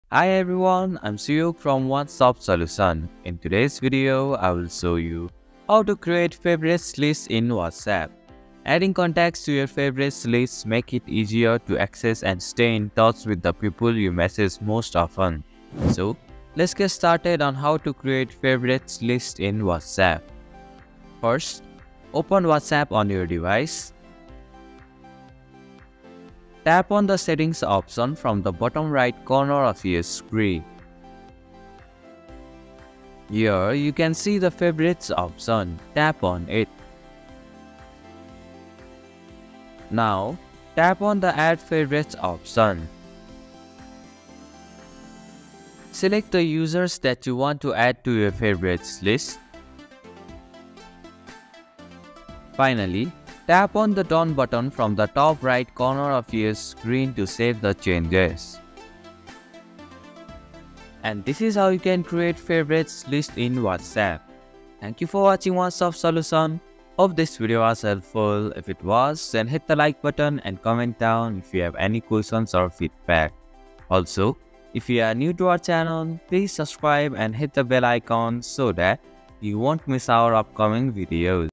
How to Create Favorites List on WhatsApp | Add Someone To Favorite List | ONE STOP SOLUTION
In this tutorial by One Stop Solution, you'll learn how to create a favorite list on WhatsApp, making it easier to connect with your most important contacts. Follow this step-by-step guide to organize your chats and prioritize messages from family, friends, or work colleagues. This tutorial is perfect for anyone looking to streamline their communication on WhatsApp and have quick access to their frequently contacted people. Watch now and simplify your WhatsApp experience!